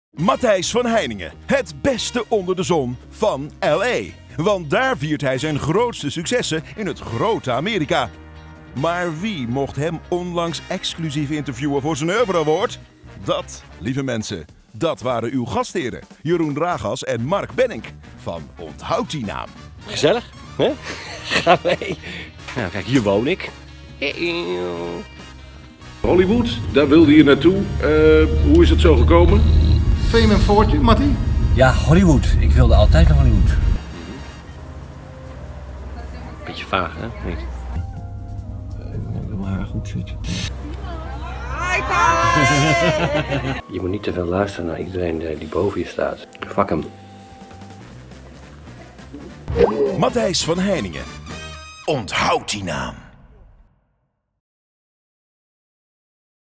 Onthoud die naam Matthijs van Heijningen
Nederlandse Hollywood regisseur Matthijs van Heijningen vertelt hoe blij hij is met zijn Nederlandse oeuvre prijs. Intro op interview.